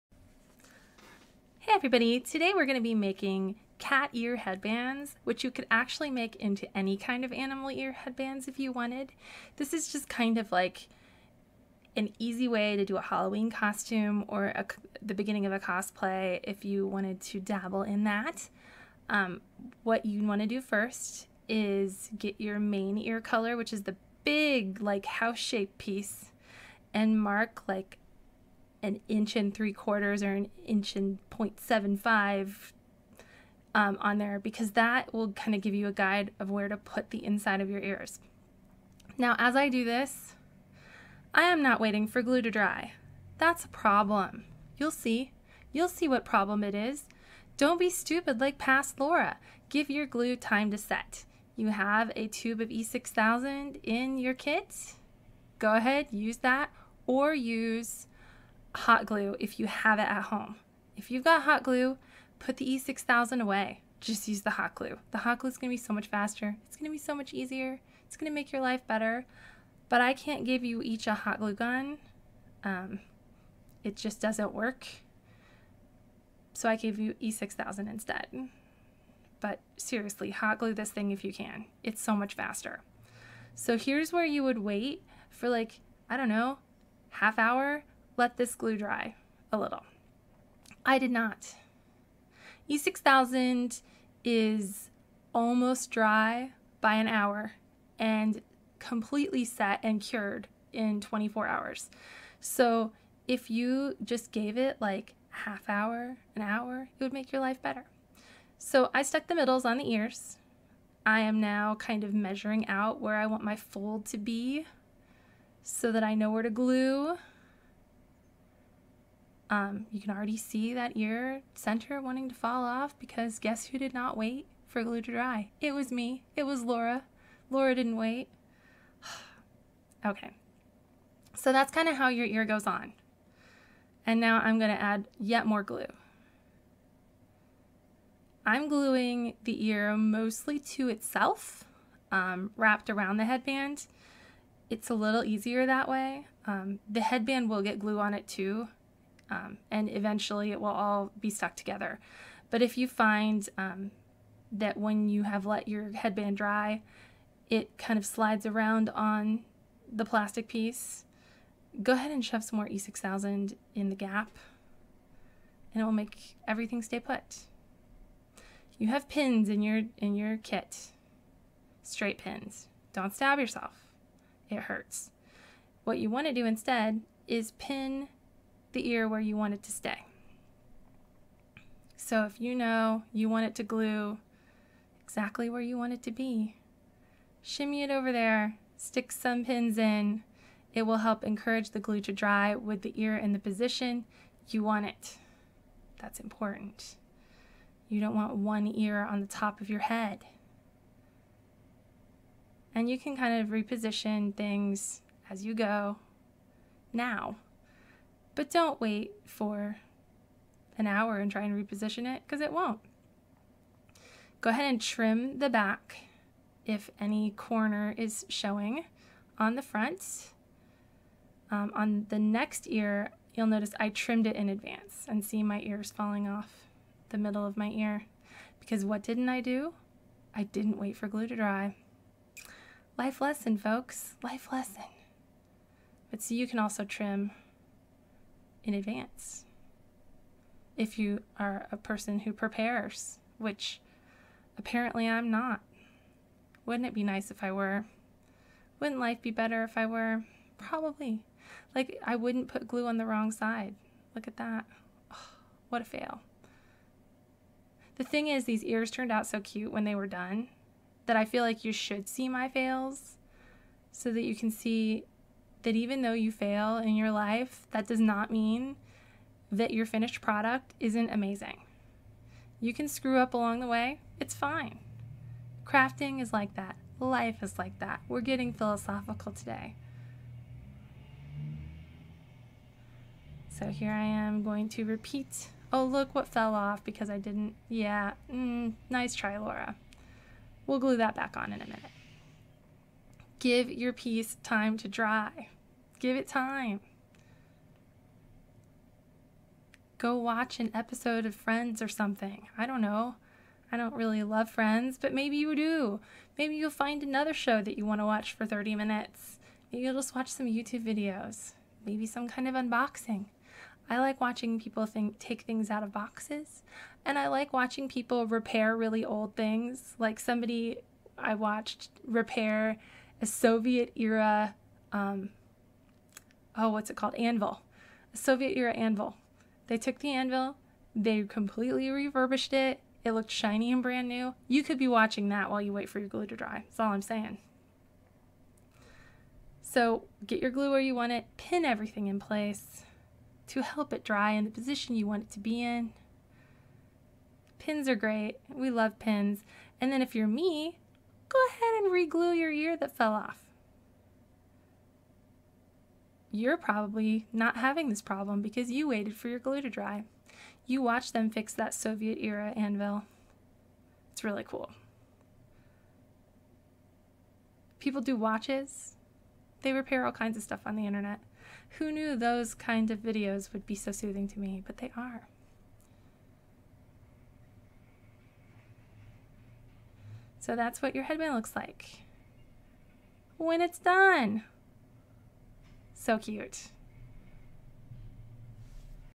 Cat Ear Headband
This week's Teen Craft Kit is here! Learn how to make a Cat Ear Headband along with Laura. Kits are free at the library for grades 6 to 12 while supplies last.